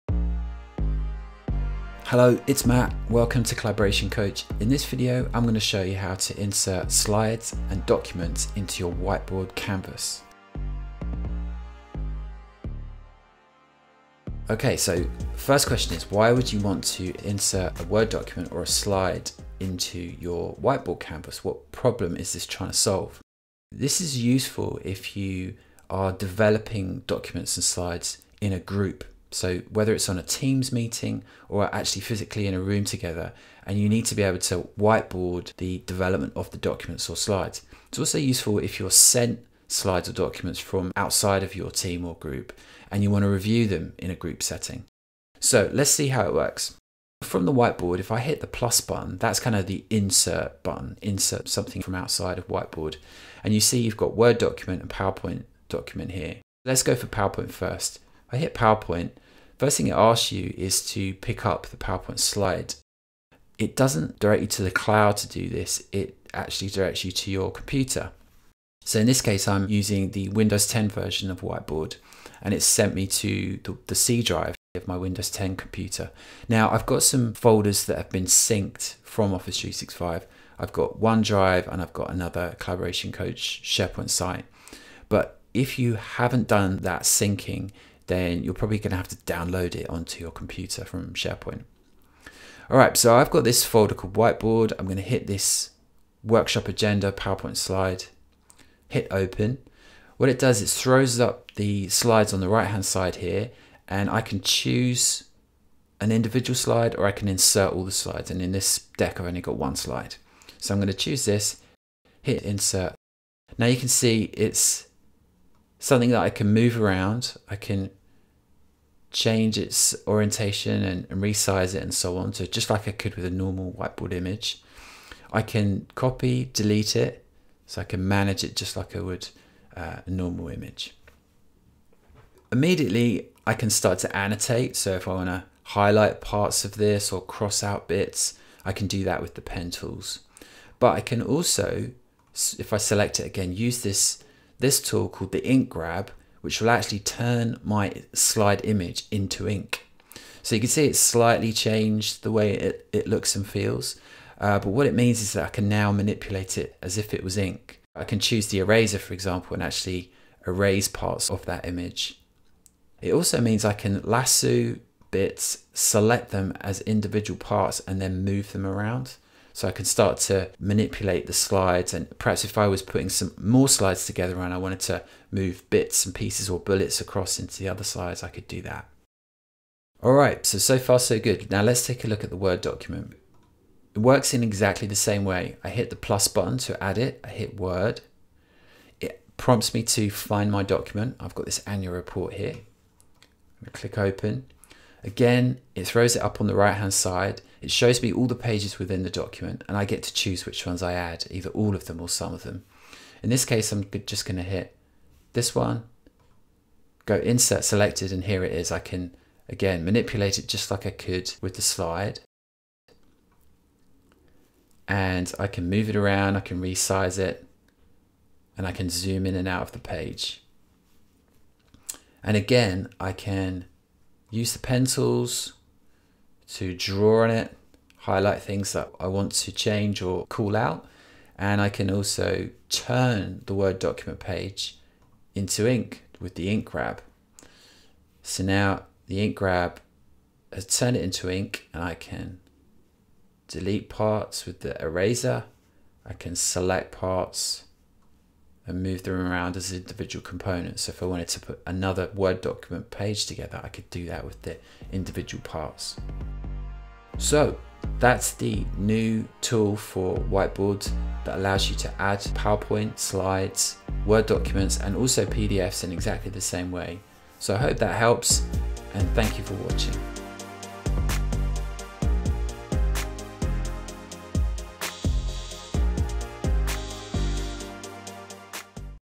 Add Word Docs and PowerPoint Slides to Your Microsoft Whiteboard
Here is a tip for Microsoft Whiteboard - you can insert PowerPoint slides, Word documents and PDFs into your Whiteboard. Useful for reviewing them in a group.